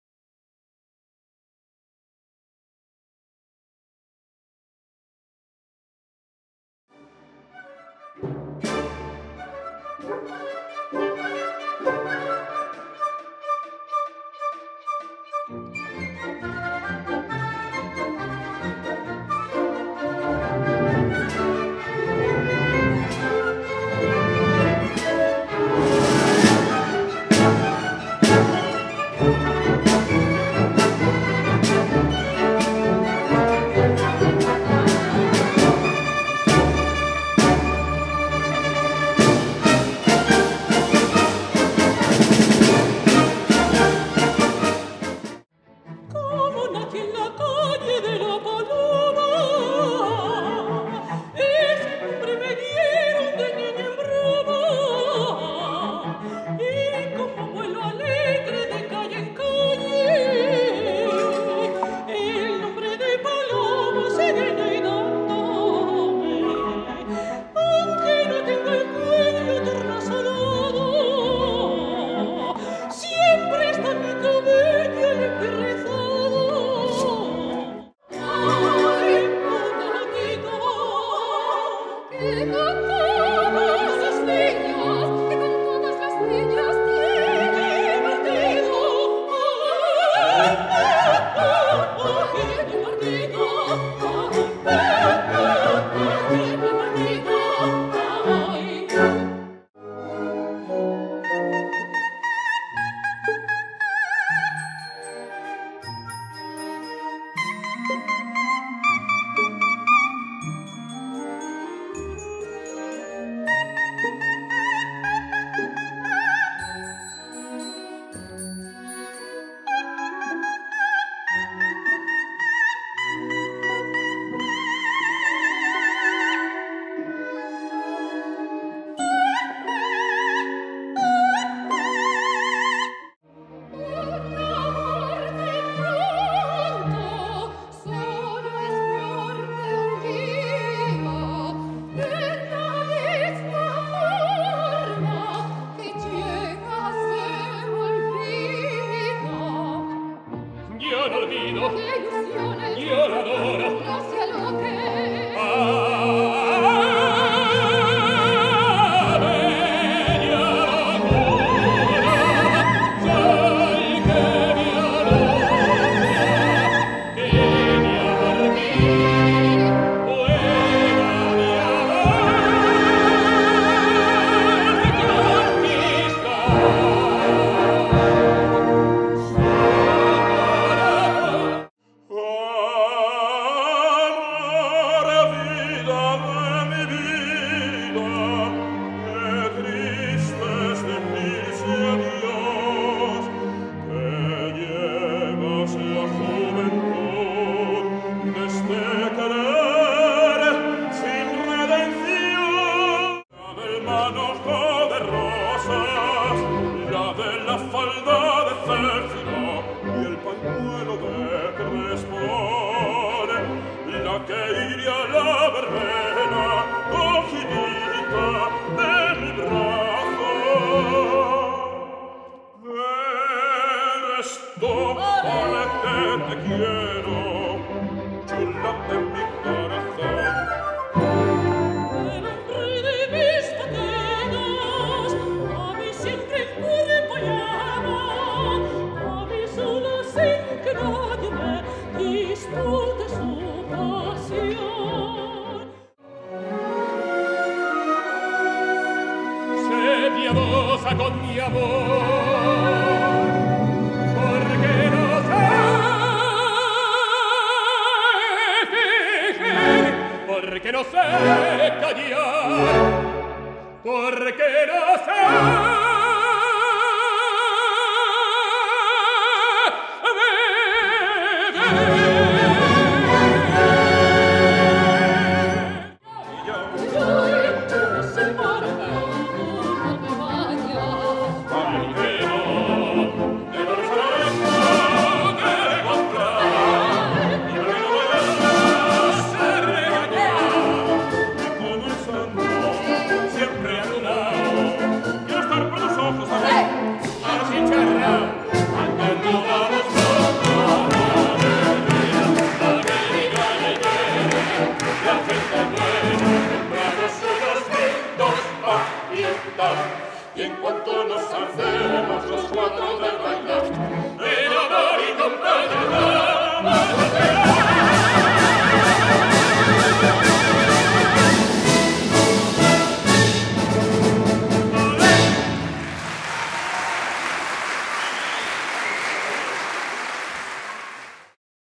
La Magia de la Zarzuela | Cía Teatral Clásicos de la Lirica, DRAO prods | DOGMA LÍRICA por i-Tek.es
Vídeo promocional del espectáculo La Magia de la Zarzuela por la Compañía Teatral Clásicos de la Lírica, DRAO producciones

INTÉRPRETES                                                                                                                                                                                                                                                                                      Margarita Marbán, soprano                                                                                                                                                                                                                                                                Milagros Martín, mezzosoprano                                                                                                                                                                                                                                                           Enrique Ferrer, tenor                                                                                                                                                                                                                                                                       Antonio Torres, barítono

ORQUESTA Iuventas

DIRECCIÓN DE ESCENA Luis Roquero
                                                                                                                                                                                                                                                   DIRECCIÓN MUSICAL Ruben Fernández
                                                                                                                                                                                                                                 ESCENOGRAFÍA César Recuenco Álvarez
ILUMINACIÓN Jose Luis Llorente                                                                                                                                                                                                                                                           COORDINACIÓN ORQUESTA María Poza                                                                                                                                                                                                                                                                                                                                                                                                                                                                                       PRODUCCIÓN DRAO Producciones SL                                                                                                                                                                                                                                             CONTRATACIÓN Sara González  Tlf. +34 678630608

PRODUCCIÓN AUDIOVISUAL
i-Tek.es | Rodaje y producción audiovisual y multimedia | Calidad 4K HD panorámico